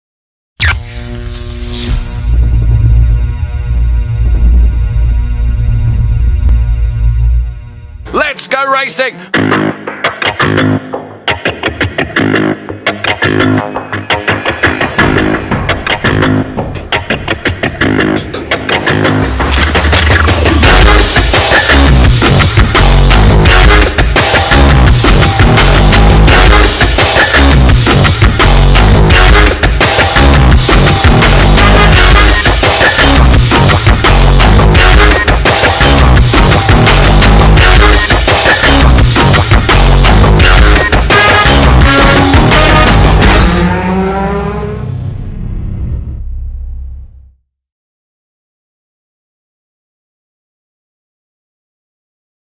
Keagan Masters official
Keagan Masters Official, sponsorship promotion video for Audi TT Cup 2017.